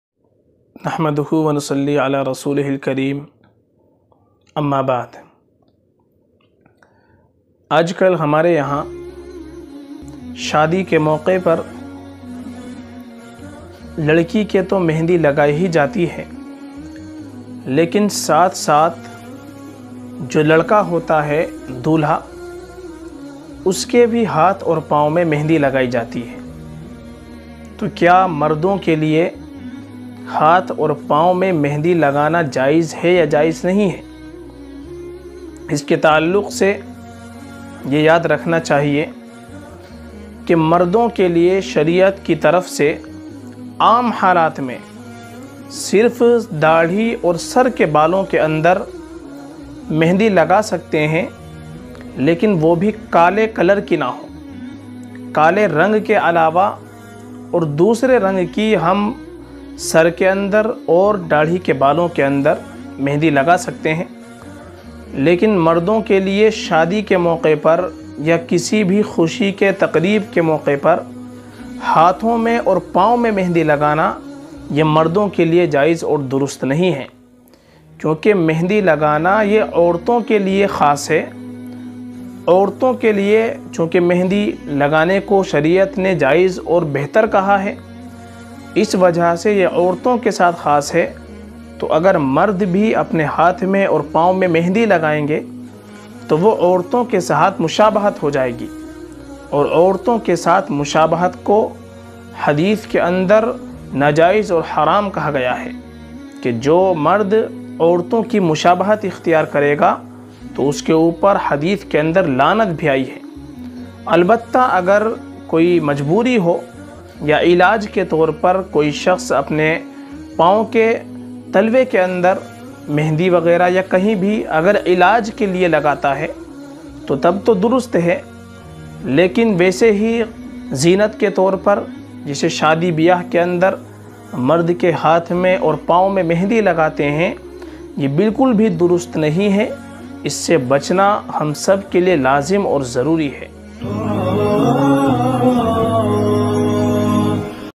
Mardon ke liye hath aur Pao me mehndi lagana kaisa hai? By Mufti Sadaqat Husain qasmi
Kya mard hath aur paon mein mehndi laga sakte hain ya nahi, kin halaat mard ke liye mehndi lagana jayiz hai, by mufti Sadaqat Husain qasmi